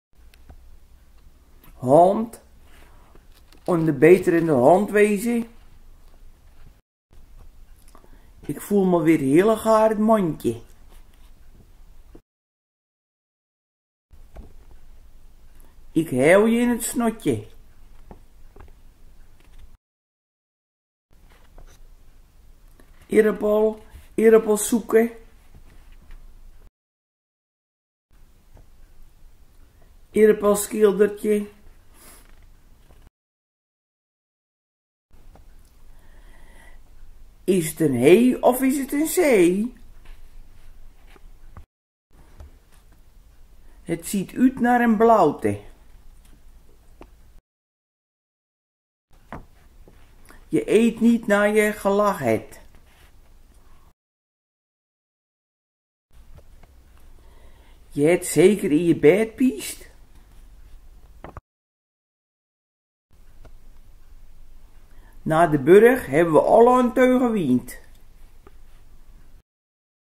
Texelse dialecten
Texelse dialect heeft een verwantschap met het West-Friese (Noord-Holland) dialect. Op onze app Wadsapp kan je nog meer zinnen en woorden leren voor je verblijf op Texel.